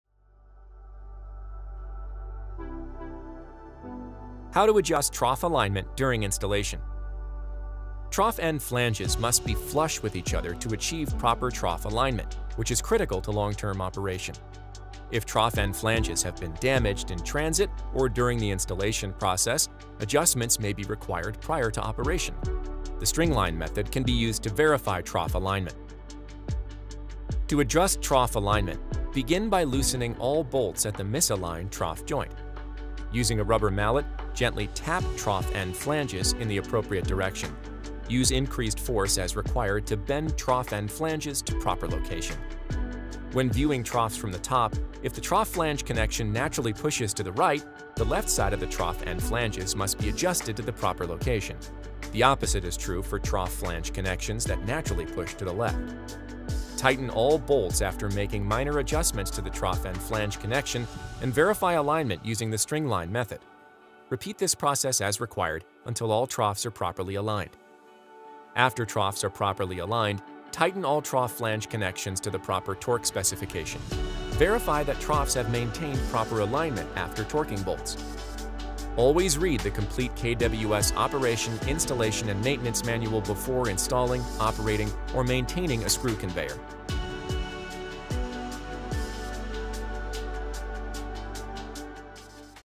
How to Adjust Trough Alignment During Installation
Screw conveyor troughs may require adjustment during installation and maintenance to achieve proper alignment. Learn how to accurately adjust misaligned troughs from the experts at KWS.